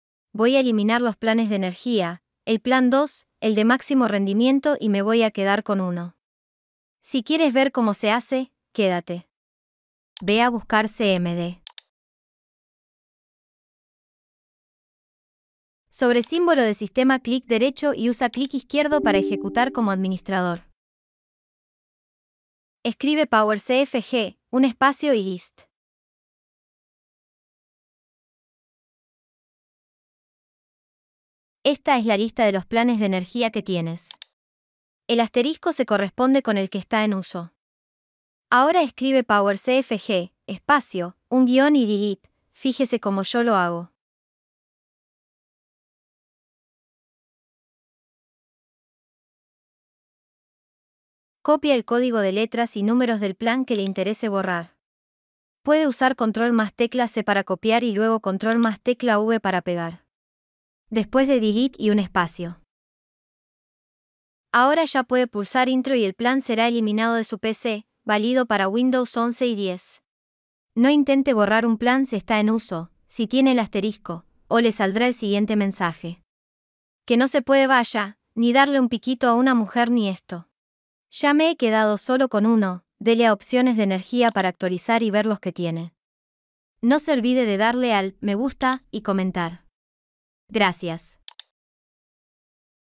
Tutorial eliminar planes de energía en Windows 11
Tutorial para quitar los planes de energía de Windows cuando no te aparece "eliminar este plan" en las opciones. Por ejemplo, puedes quitar el plan de máximo rendimiento, equilibrado, o el de ahorro de energía.

Los planes de energía en una PC con Windows son configuraciones predefinidas que controlan cómo el sistema gestiona el consumo de energía. Estos planes permiten ajustar el equilibrio entre el rendimiento del sistema y la eficiencia energética. Por ejemplo, un plan de "Alto rendimiento" priorizará el rendimiento máximo, mientras que un plan de "Economía de energía" reducirá el rendimiento para ahorrar energía. Puedes elegir entre diferentes planes según tus necesidades y preferencias, o incluso personalizar configuraciones específicas dentro de cada plan.

Estás eliminando varios planes de energía en tu computadora. Abre el Símbolo del sistema como administrador, escribe "powercfg list" para ver la lista de planes. Luego, usando "powercfg -delete" seguido del código del plan que deseas eliminar, puedes hacerlo. Asegúrate de no borrar el plan en uso (marcado con un asterisco). Luego, actualiza las opciones de energía para ver los cambios. Recuerda no borrar el plan en uso y dar "me gusta" y comentar.